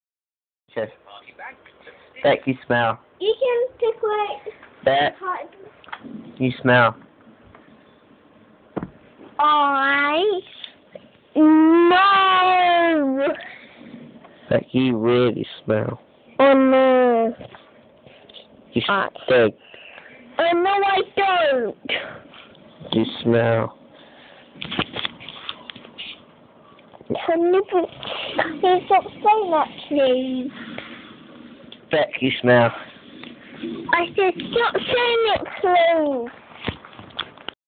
CHILD ADMITS THEY SMELL!
I were messing around with her and she started saying she smelt and I cought her! And after she ran to her mum and I got into trouble!